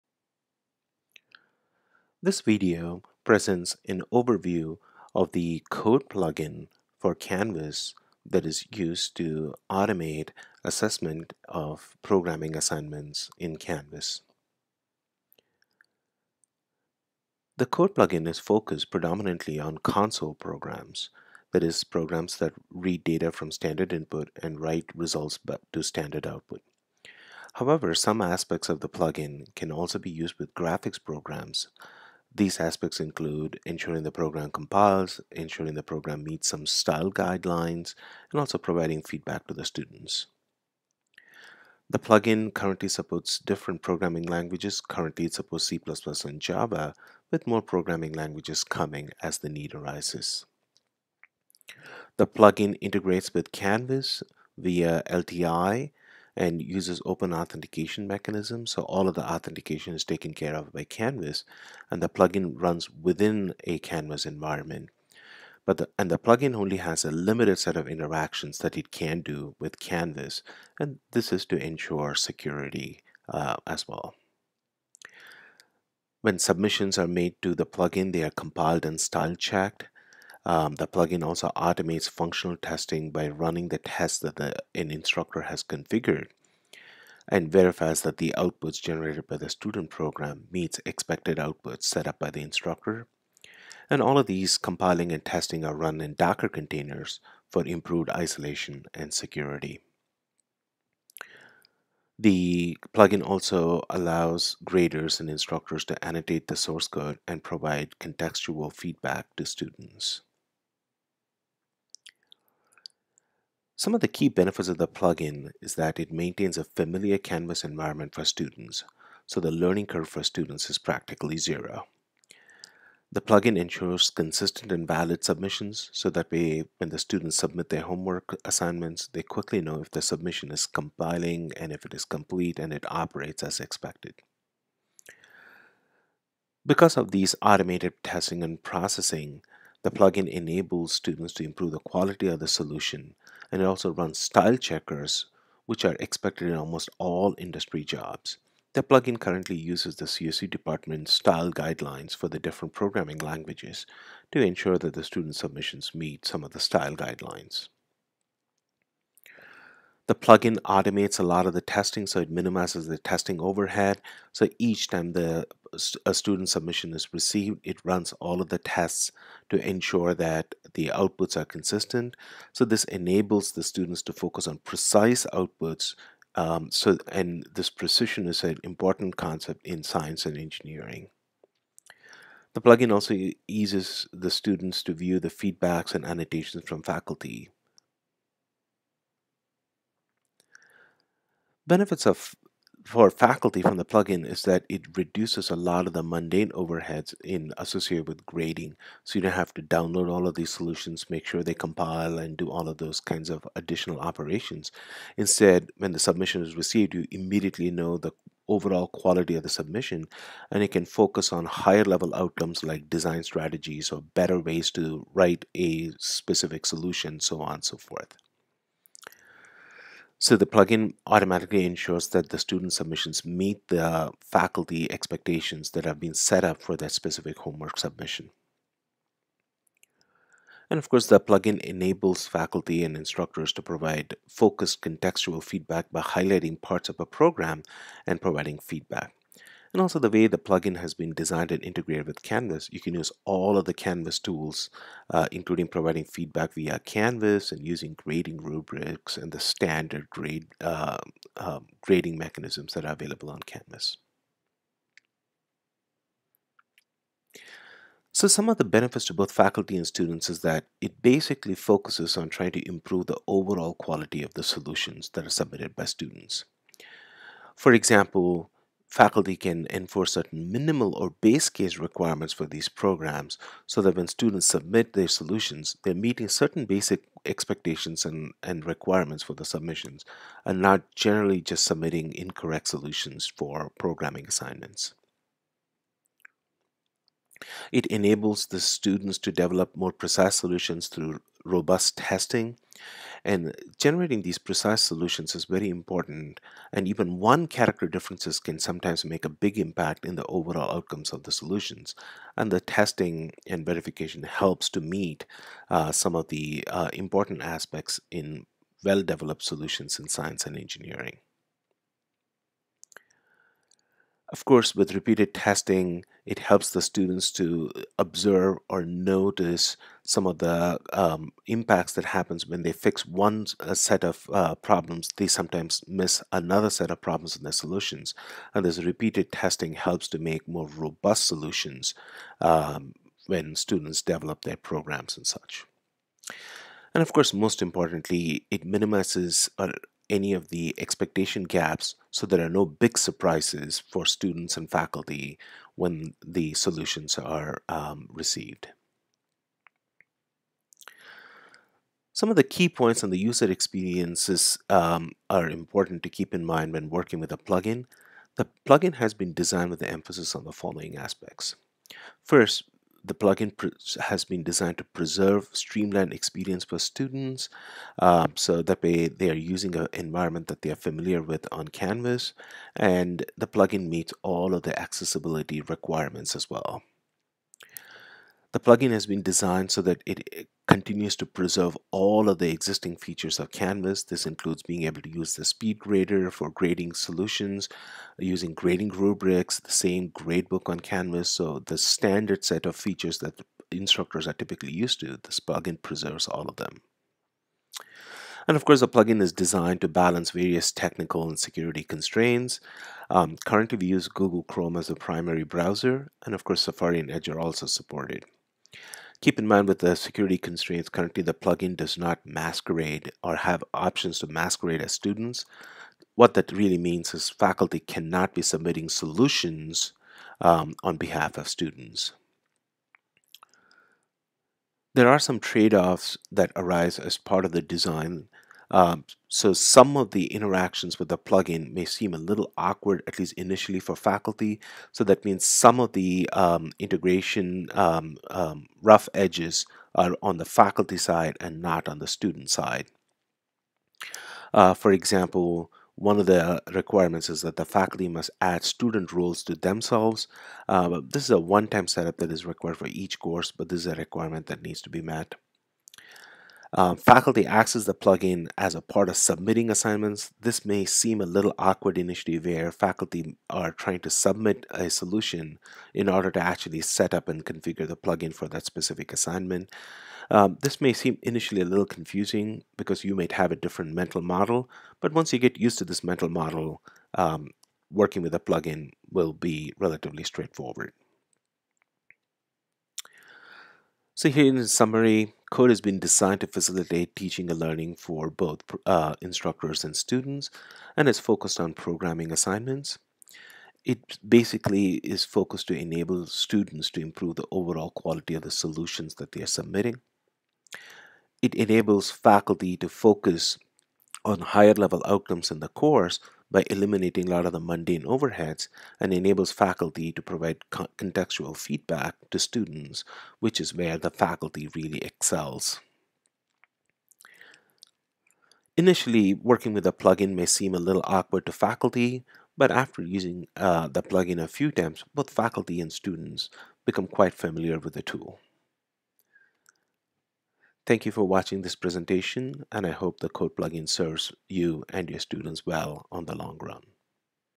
Overview of CODE LTI plug-in
This video presents an overview of the pedagogical and technical motivations for developing and using the CODE plug-in for automated assessment of programming assignments in Canvas.